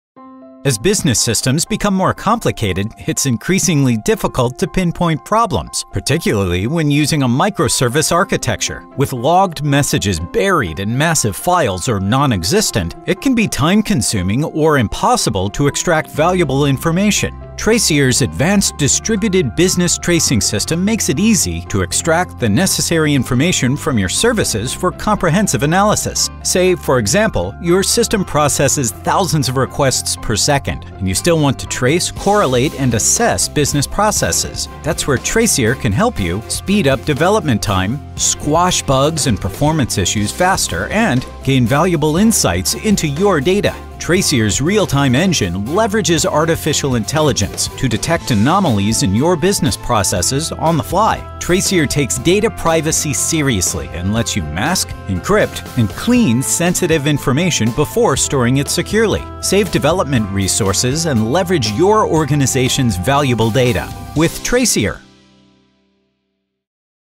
Explainer Video for Trasier | Motion Graphic Animation
This animated explainer video did a great job of demonstrating how it can be time-consuming or impossible to extract valuable information when using a micro-service architecture with logged messages.

In one full-minute video, Trasier has successfully delivered its business model solution.

Trasier is a distributed business tracing platform that gives a quick insight into data exchanged by software services. Unlike a typical APM, Trasier focuses on business tracing and detects anomalies in business processes.

Elevate your product with fully customized animated explainer video content.